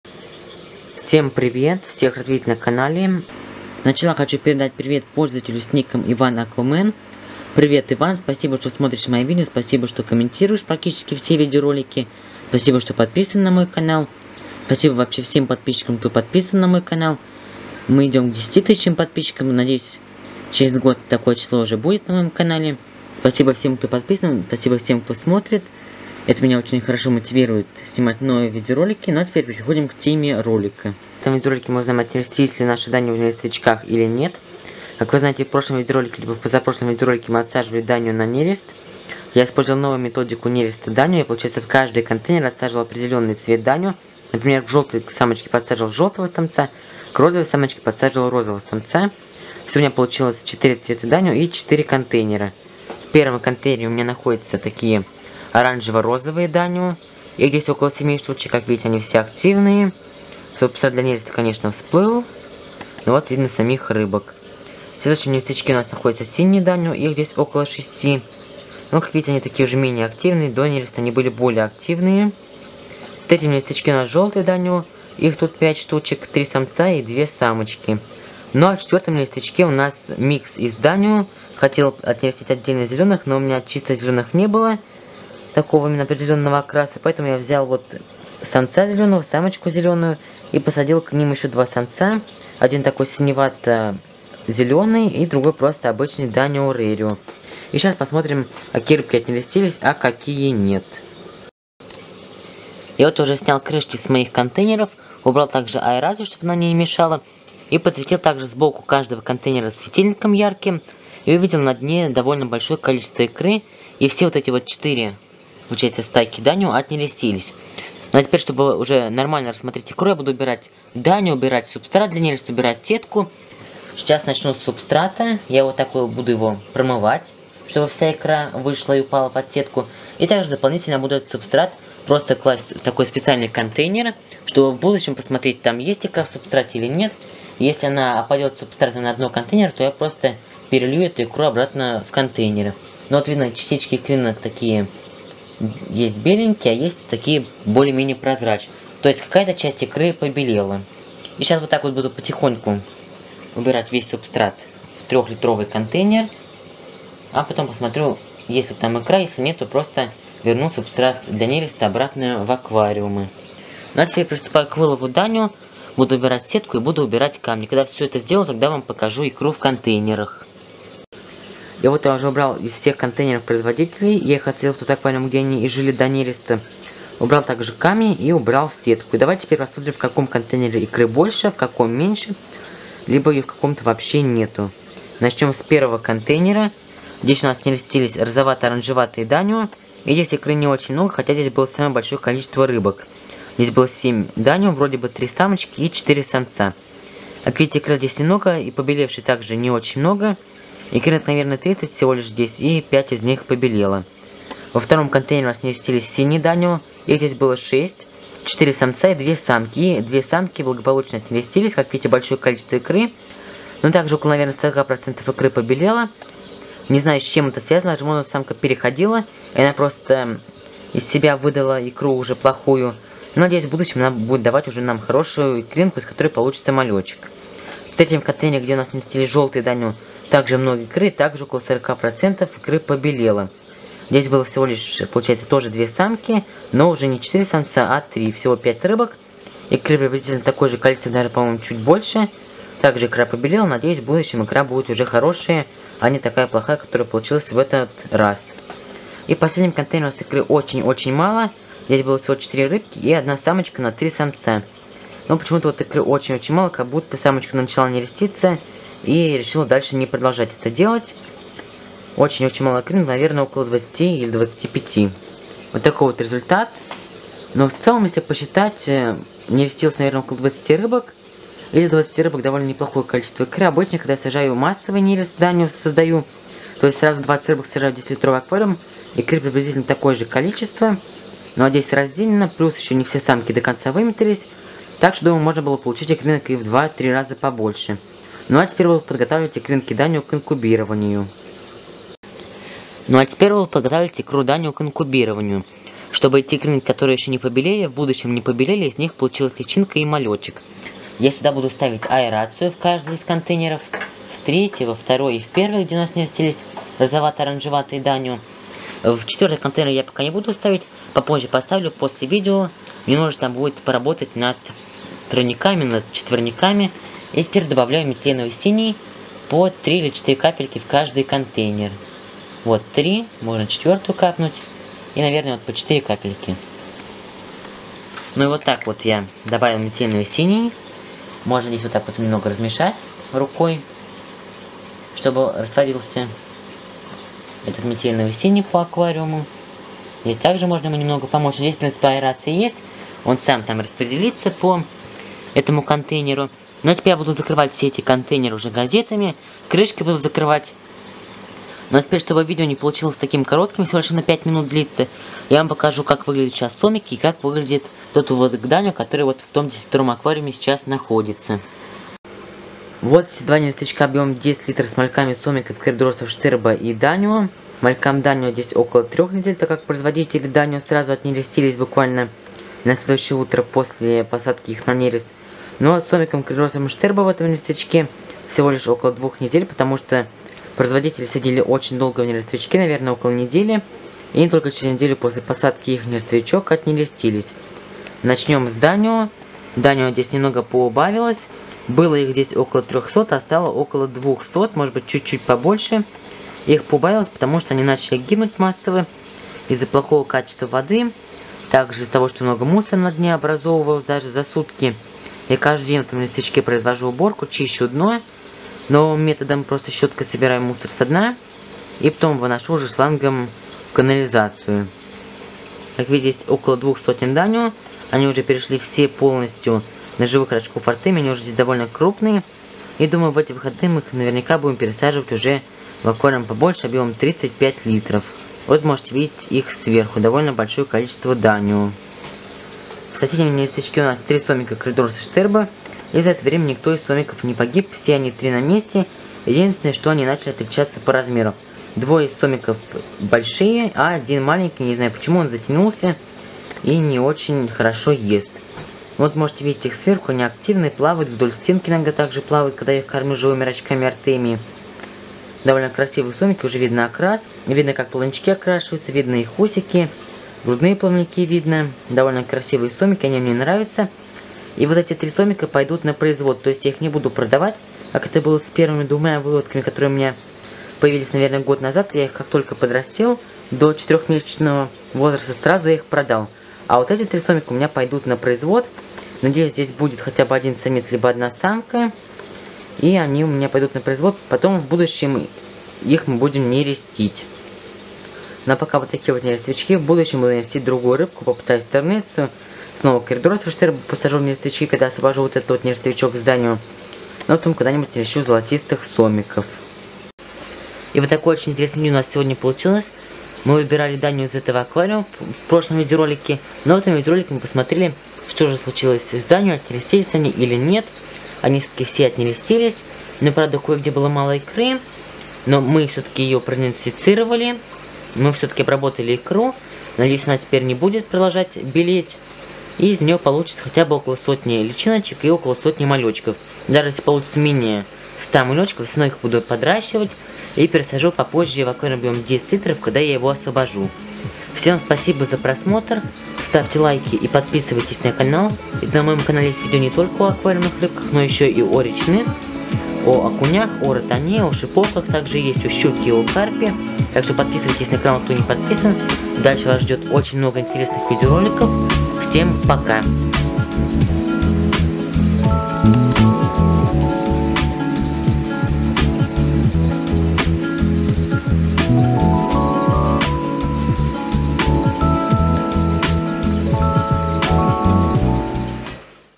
Результаты нового способа разведения данио!
В прошлом видеоролике я нерестил данио новым способом! А в этом видео мы узнаем, что из этого получилось! А получилось так, что все стайки данио отнерестились и в будущем я буду разводить данио только таким способом!